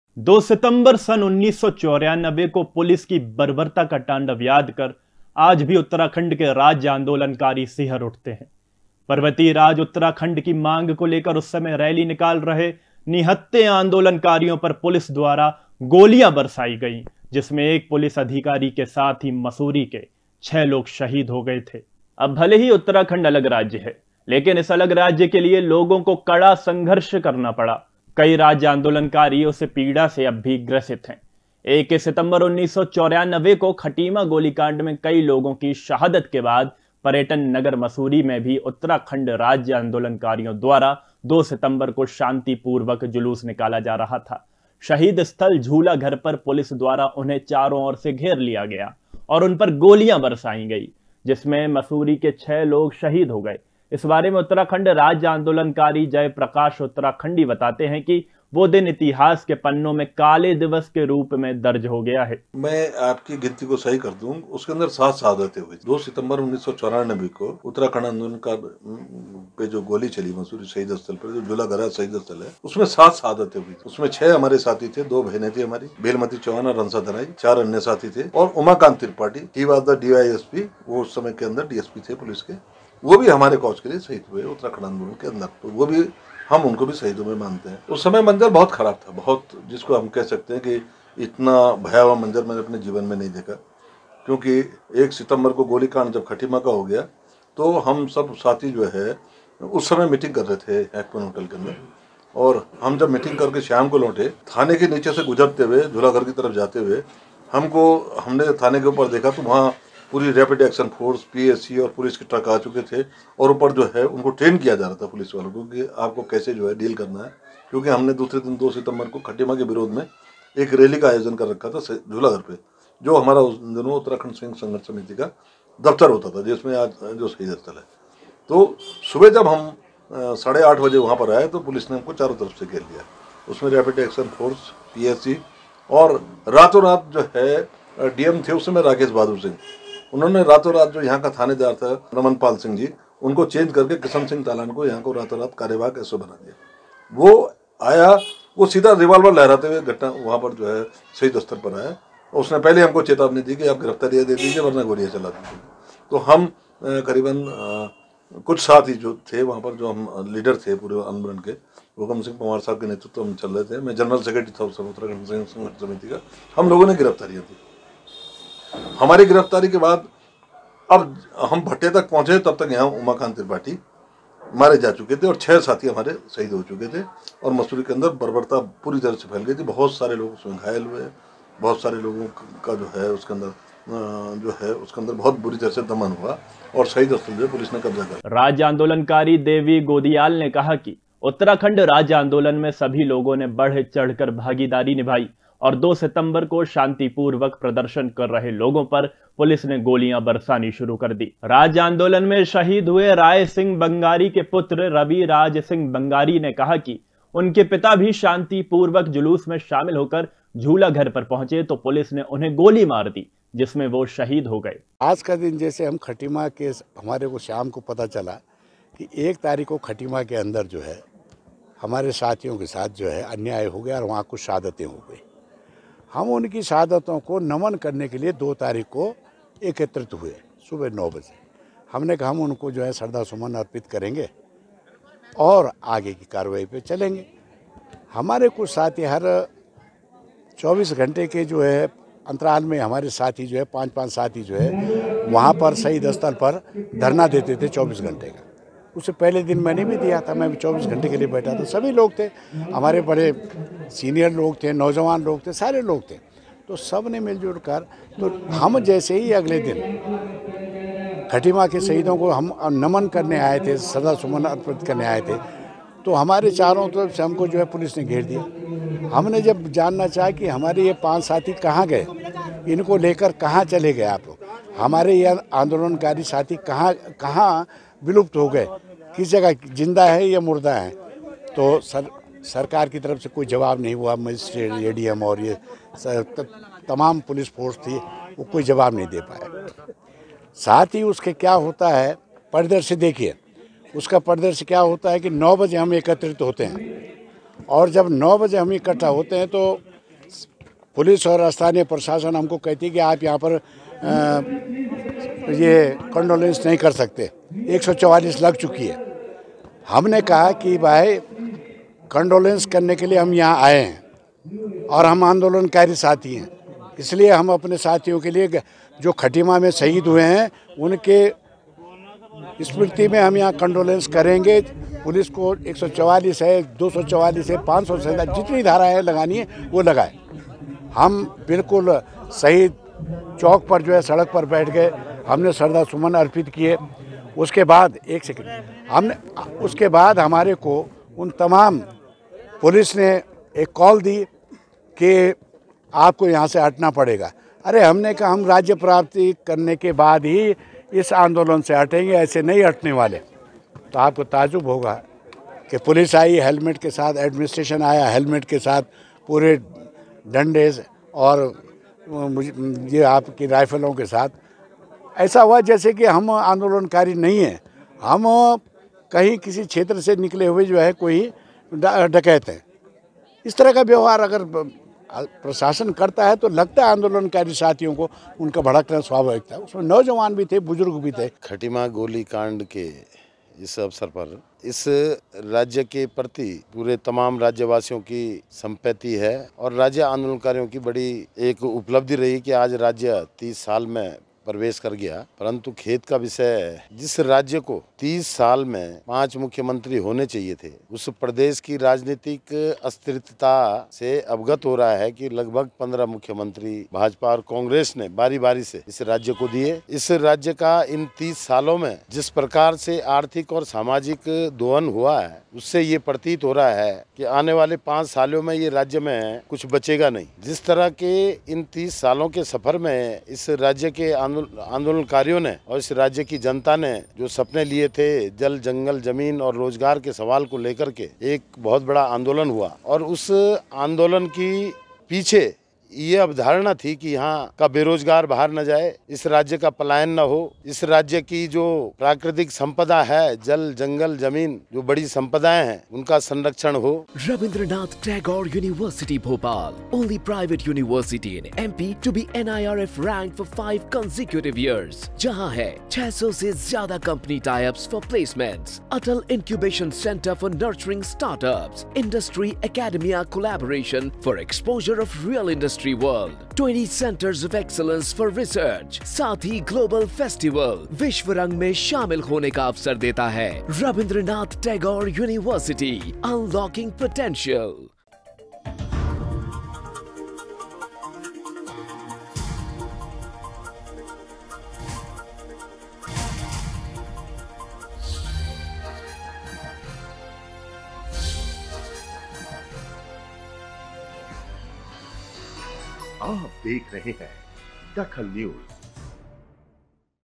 DAKHAL NEWS पुलिस की बर्बरता की यादों का तांडव
पुलिस की बर्बरता की यादों का तांडव
आज भी सिंहर उठते हैं राज्य आंदोलनकारी